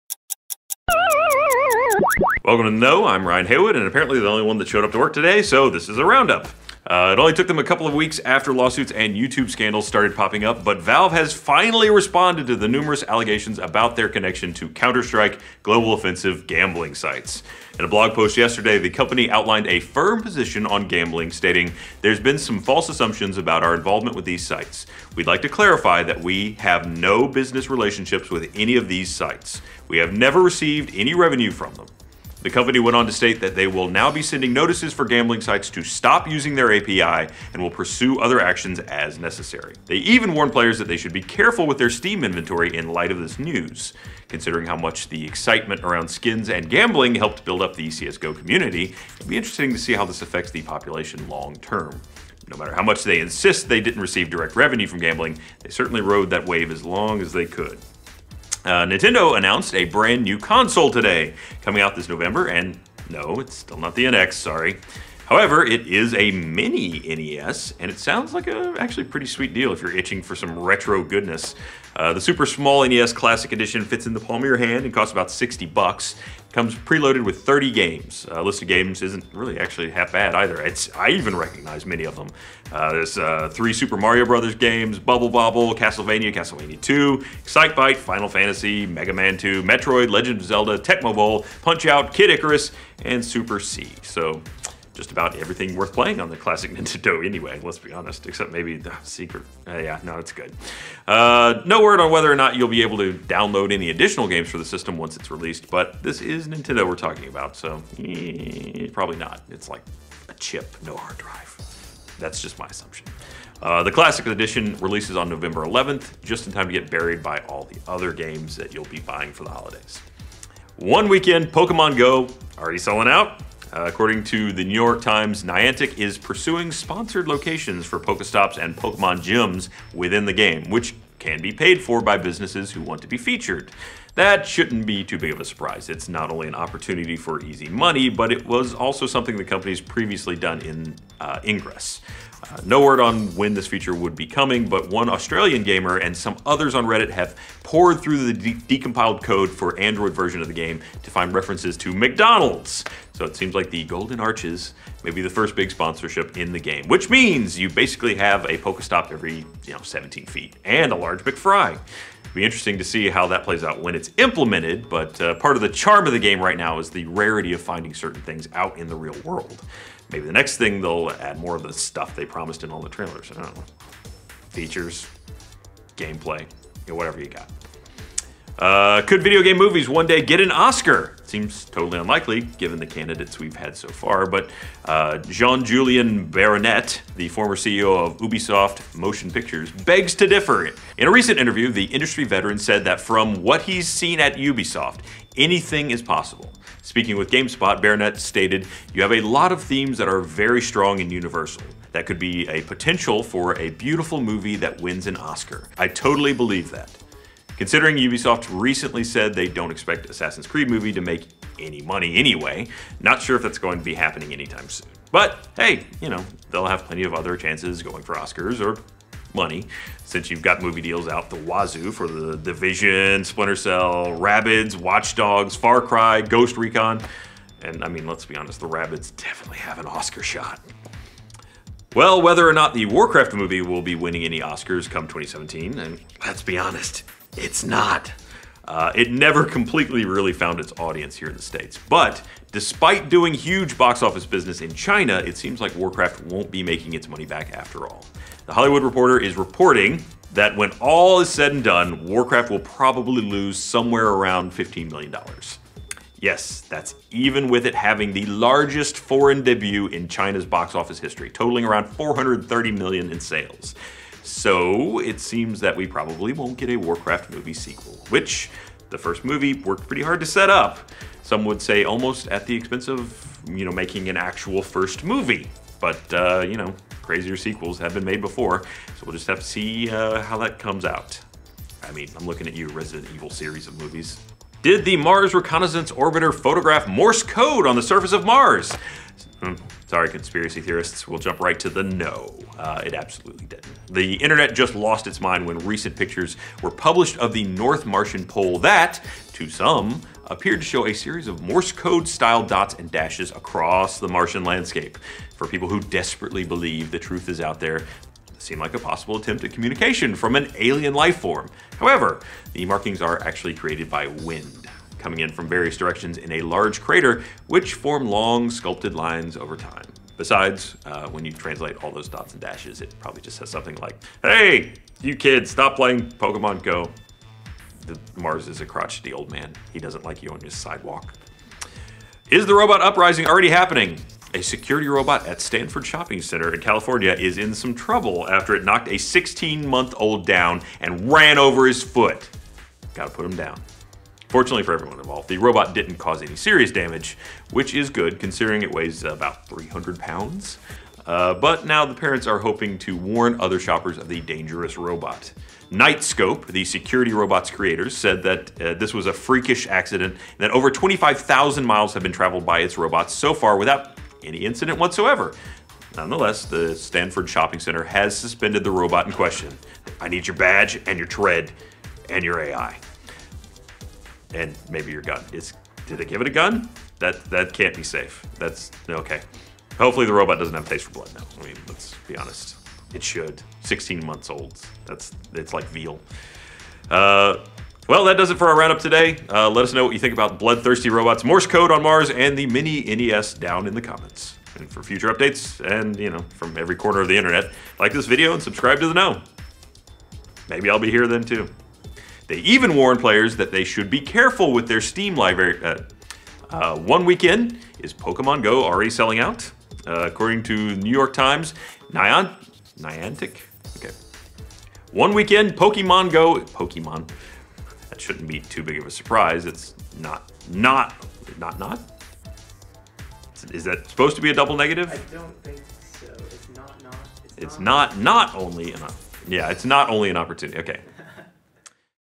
Valve Says No More Gambling + Pokemon GO Sells Out? + Robot Runs Over Kid - The Know
Valve finally responds to the CSGO gambling allegations, Nintendo announces a new console (it's not what you think), and Pokemon GO is putting Pokestops up for sale? All this new and more in today's round-up.

Hosted By: Ryan Haywood